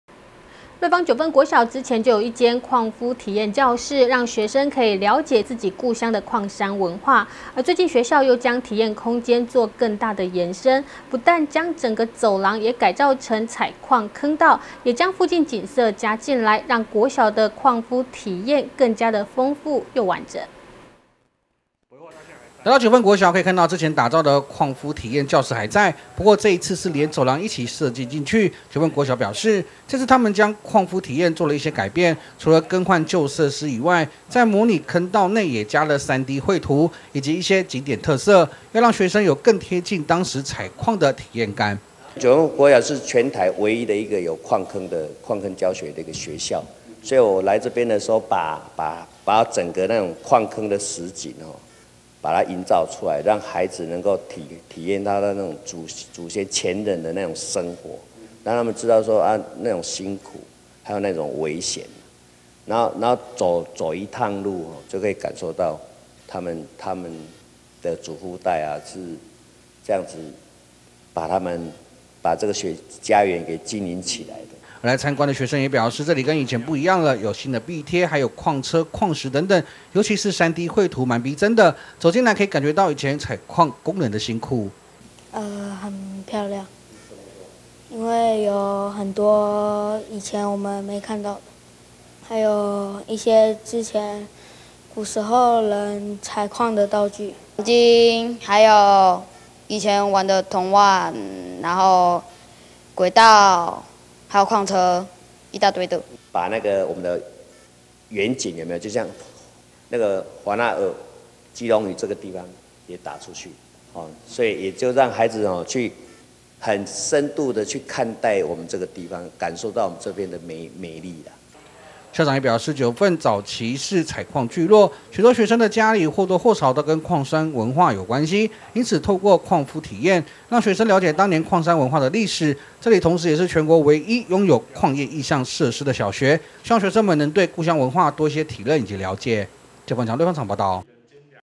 1070918新北新聞網09 瑞芳區礦夫體驗加3D彩繪 學生貼近在地文化
瑞芳九份國小之前就有一間礦夫體驗教室，讓學生可以了解自己故鄉的礦山文化，最近學校又將體驗空間作更大的延伸，不但將整個走廊也改造成採礦坑道，也將附近景色加入進來，讓國小的礦夫體驗更加豐富又完整。